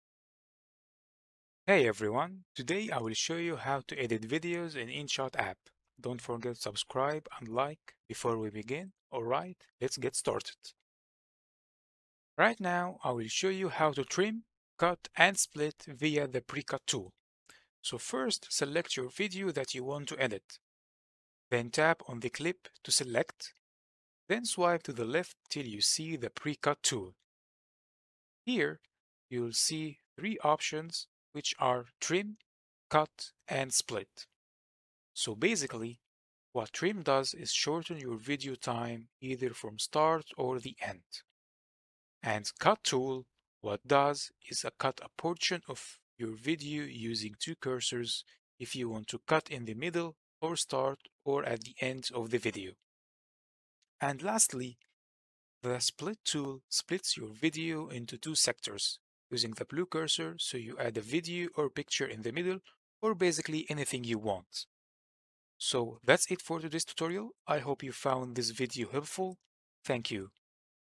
How To Edit Video In Inshot App (2024) Full Tutorial
How To Edit Video In Inshot App. It is really easy to do and learn to do it in just a few minutes by following this super helpful tutorial to get a better understanding of everything.